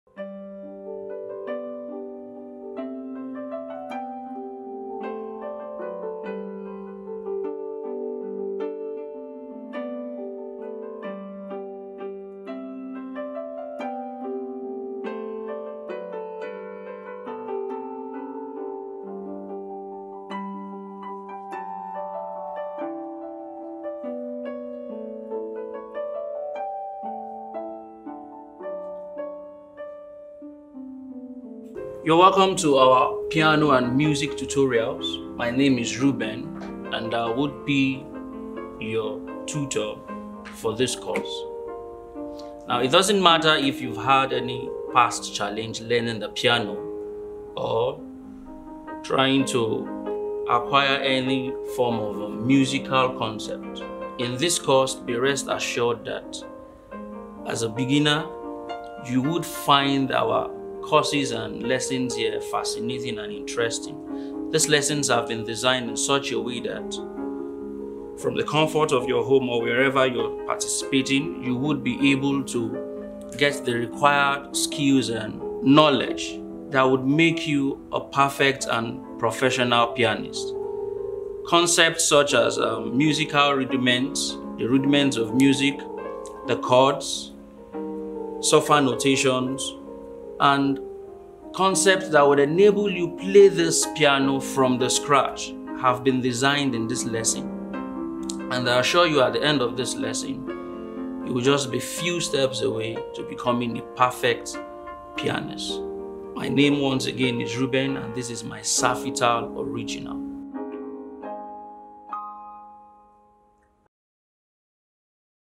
Piano and Music Tutorials for Beginners [ Trailer ]
Music is a powerful force. Knowing how to use this force is a dream for most people. This course offers a full range of simplified lessons for all ages. Modules have been designed in a style that caters to your musical interest and will help unleash your creativity. Get that piano classes you have always wanted at the tip of your fingers,